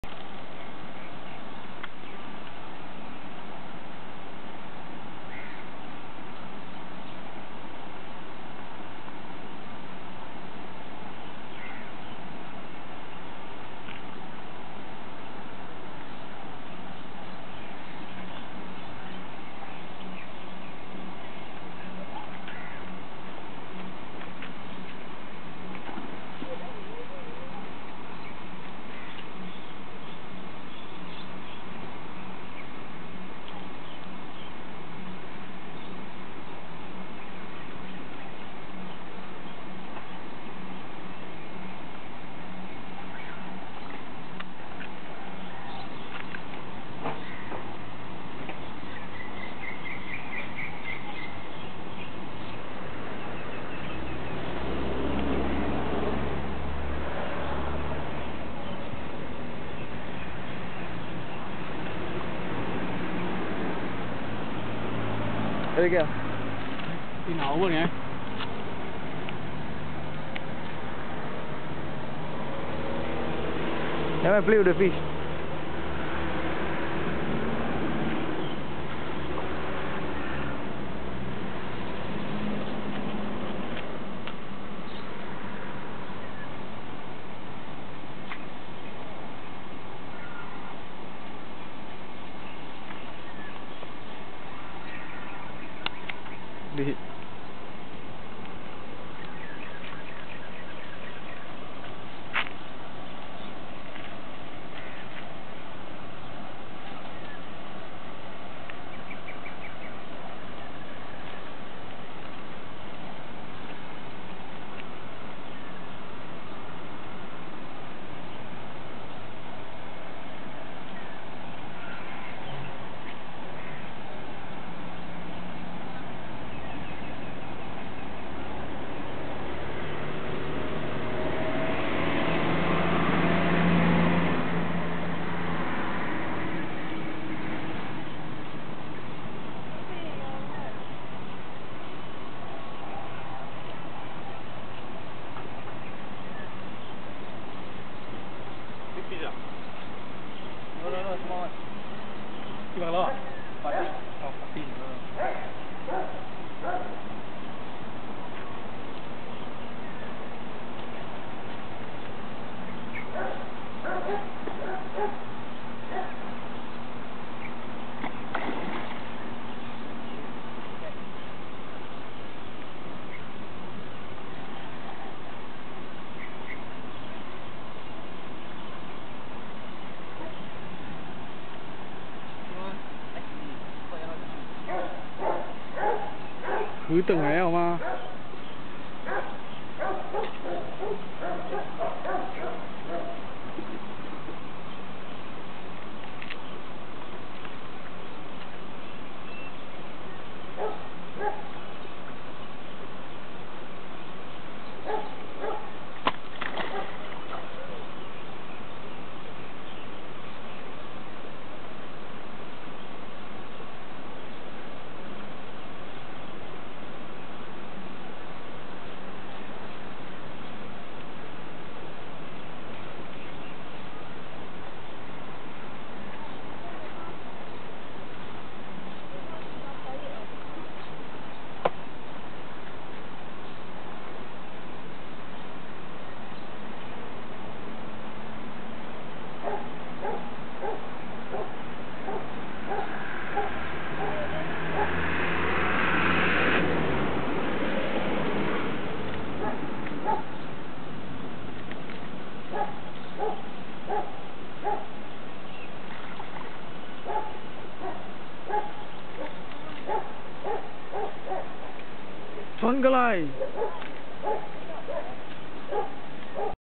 P@tin Fish
Dear2 second fish caught on her new set of tackle...